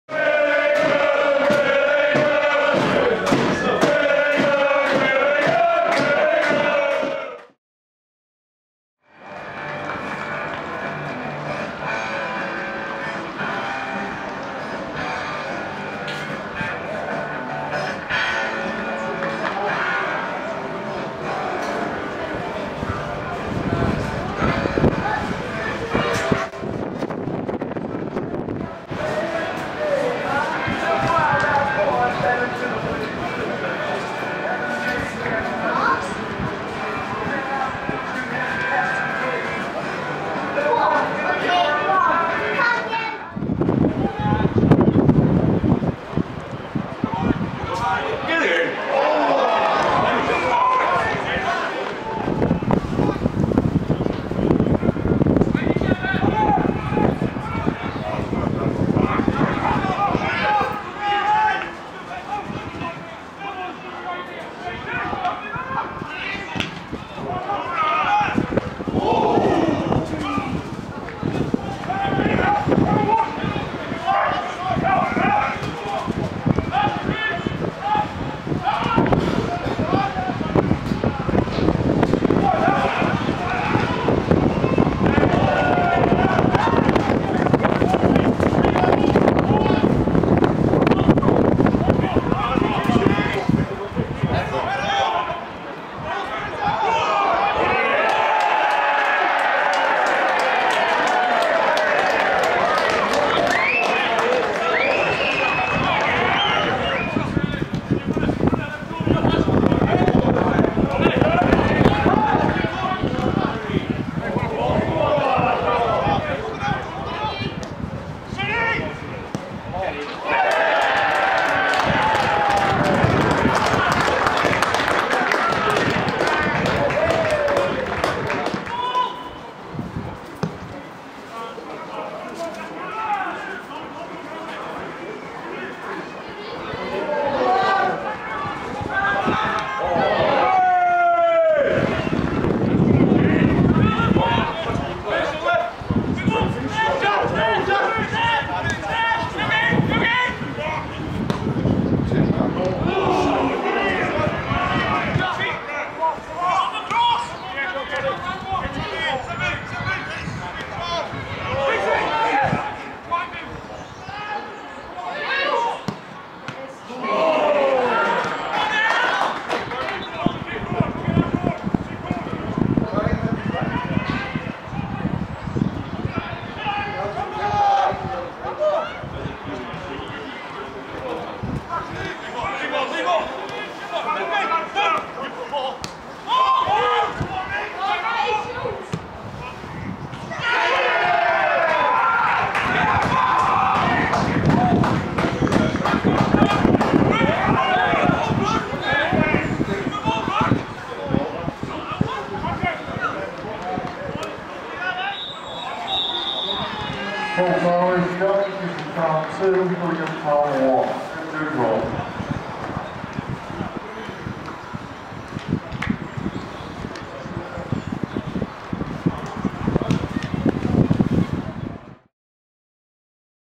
Guisborough Town FC 2 - 1 Billingham Town FC
Highlights from our 2-1 away defeat to Guisborough Town FC at the King George V Ground in the EBAC Northern League Division 1 on Saturday 1st February 2020.